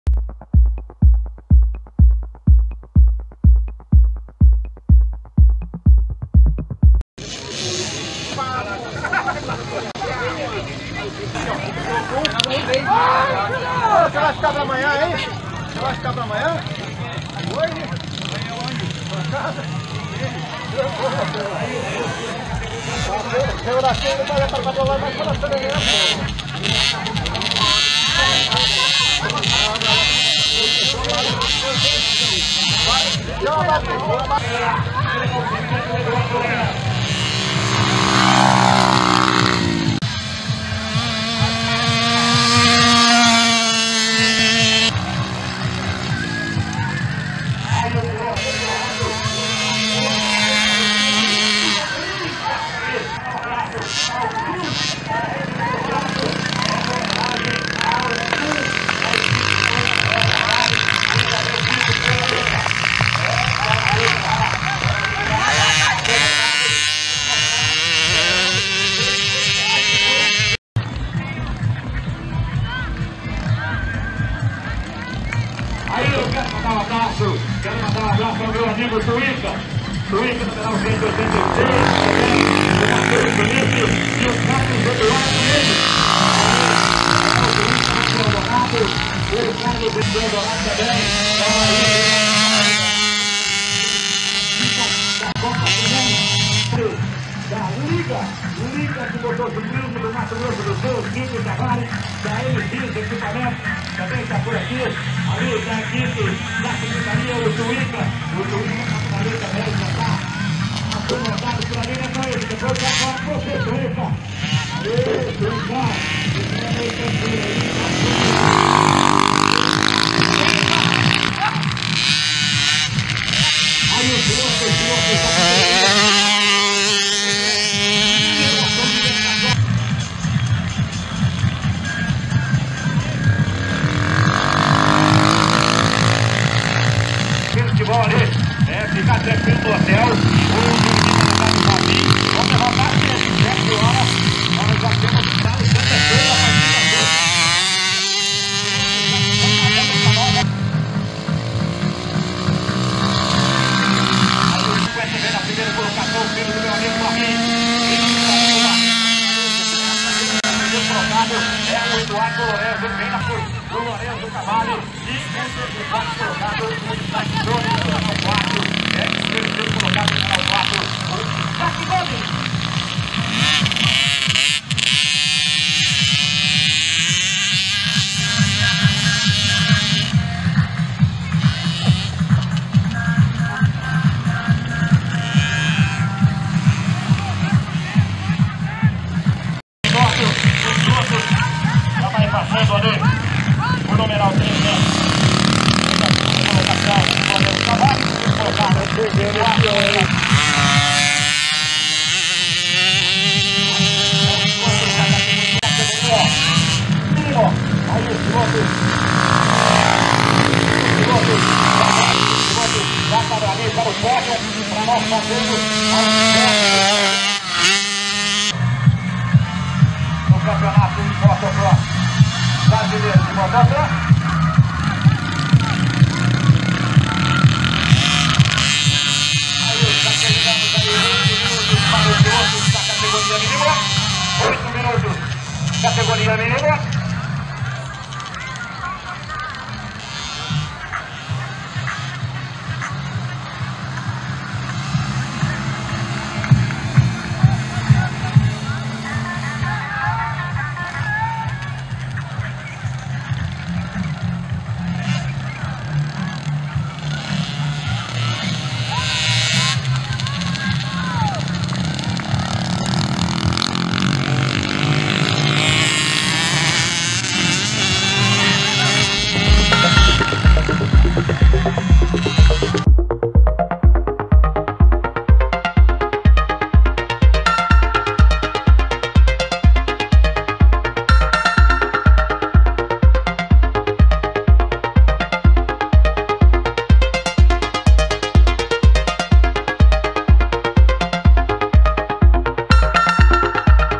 Super Mini em Aquidauana pela Copa 4B Sul-mato-grossense de Velocross 2024
Essa produção ocorreu graças aos parceiros do Blog Holeshot:

Ipê Dourado Restaurante
Columbia Medicina
Laboratório Bonito
Letícia MX
Box57
Auto Giro Motos
Royal BetPro

Evento realizado em Aquidauana (MS) no domingo (03).